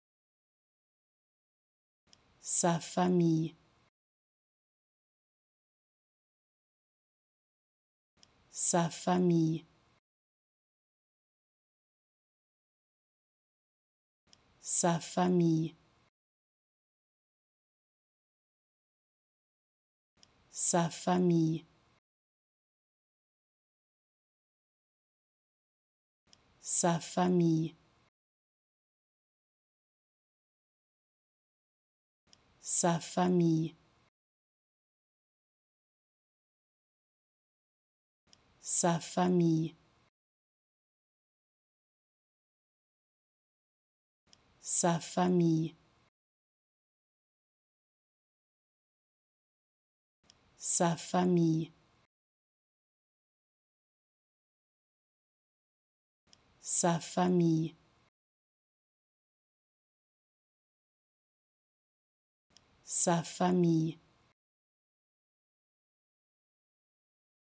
How to Pronounce ''Sa famille'' (His family) Correctly in French
Learn how to say and properly pronounce ''Sa famille'' in French with this free pronunciation tutorial. Apprenez à prononcer des mots en français grâce à nos tutoriels.